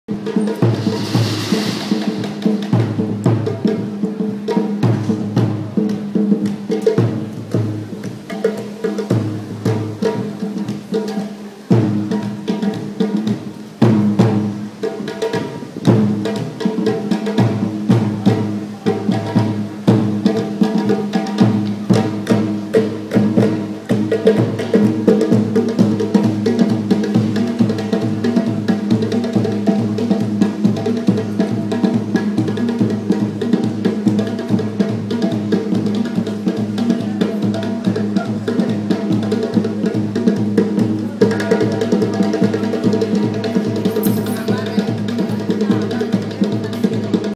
Napoli: i tamburi del Vomero
Una vera e propria prova di resistenza di fronte a questi rumori assordanti e continui nella centralissima via Scarlatti. L'ordinanza sindacale che disciplina l'attività' degli artisti di strada e' scaduta il 31 ottobre scorso, ma chi dovrebbe far rispettare la disposizione non lo fa e questo è il risultato.